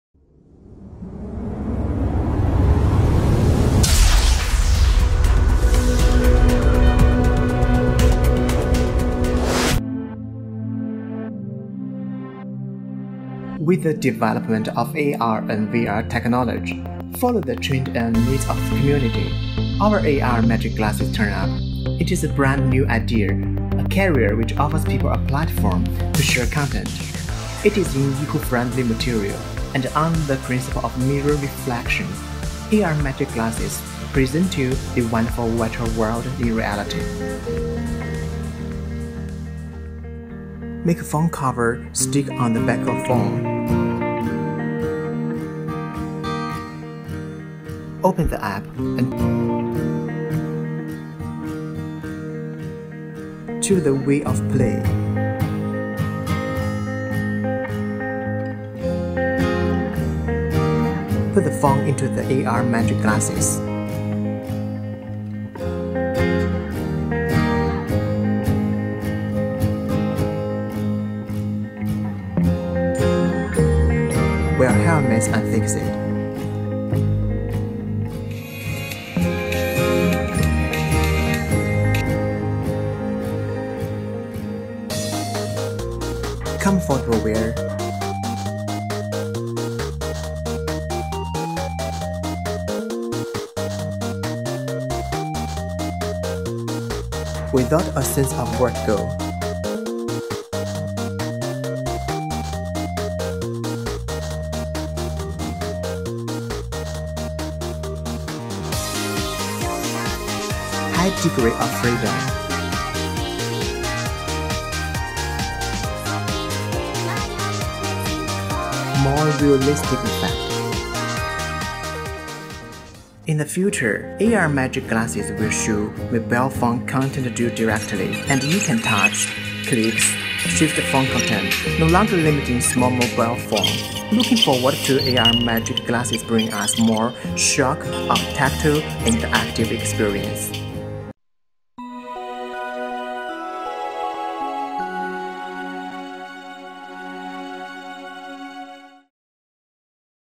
World's creative augmented reality AR box magic ar headset glasses
— — Independent research and development magic augmented reality AR box which makes the Whole World Your Aircraft-Defense Battlefield.
 — — Perfectly compatible with Android and IOS mobile phone system,and you can choose between playing by phone or helmet,and play with 360° panoramic or AR card.
 — — No sense of vertigo or uncomfortable experience due to our Ergonomics design and
advanced technology.
 — —The combination of Virtual and realistic bring you the true holographic experience wherever you are.